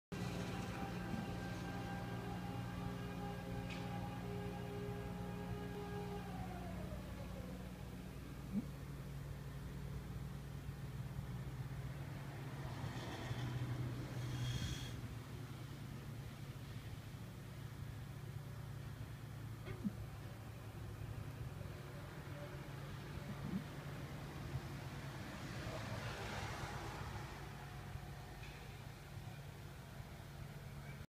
Spinne tötet Marienkäfer
Hier könnt ihr die Vorgehensweise einer Spinne bei der Jagd beobachten :D